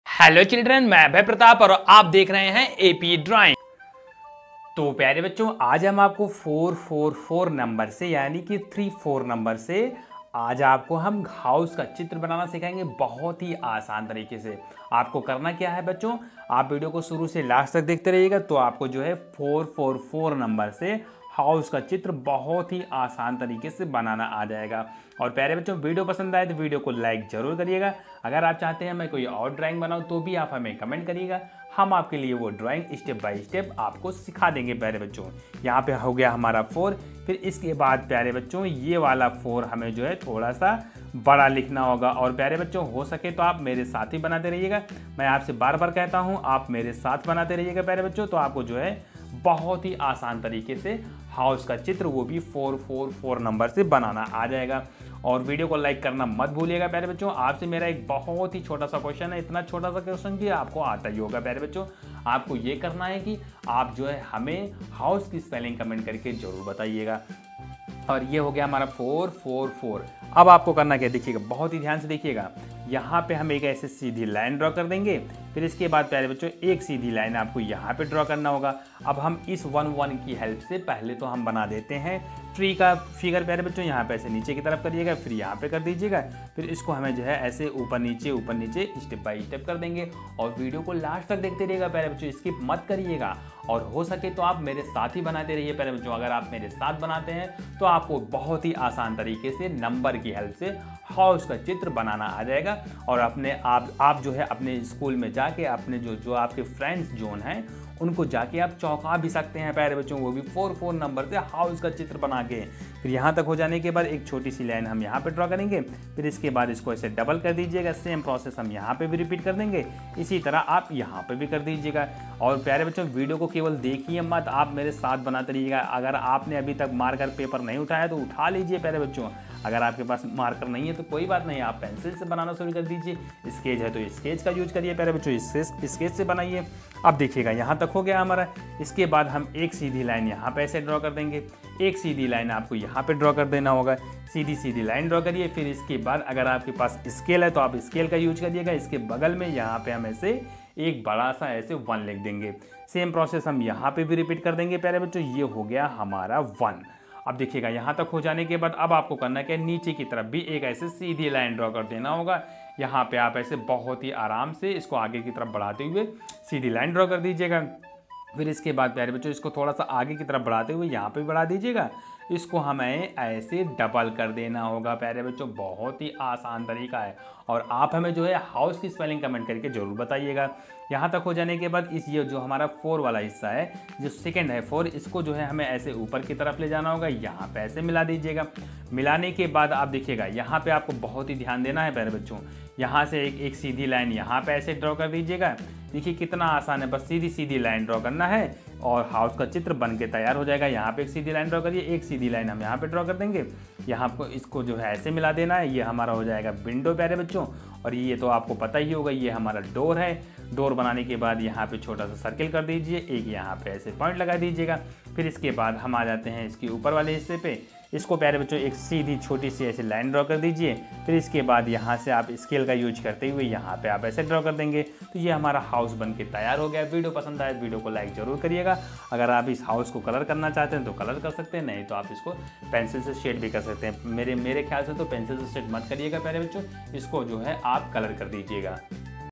घर का चित्र बनाना सीखें | how to Draw house step by step | AP Drawing
today I am Drawing how to Draw house step by step doodle art on paper learning Drawing